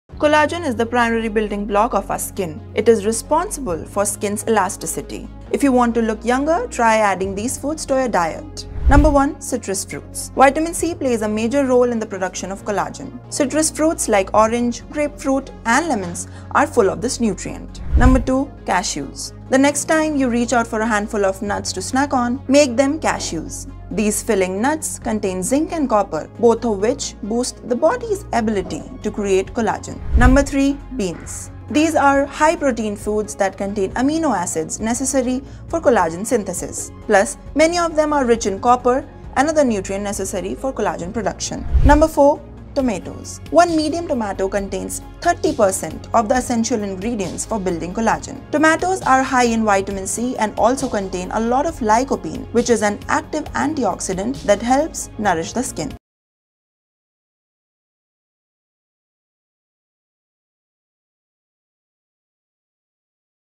Skin Care : Want glowing skin naturally? Eat these foods daily to boost collagen in the skin
Collagen is a protein that serves as a crucial building block in our skin, making up a significant portion of its structure. This protein provides strength, elasticity, and firmness to the skin, contributing to its overall health and appearance. As we age, the production of collagen decreases, leading to wrinkles, sagging, and a loss of skin elasticity. Boosting collagen is essential for maintaining youthful and radiant skin, and it can be achieved through a healthy diet, skincare routines, and lifestyle choices. Incorporating collagen-boosting foods and practices helps support the skin's structure and promote a vibrant complexion.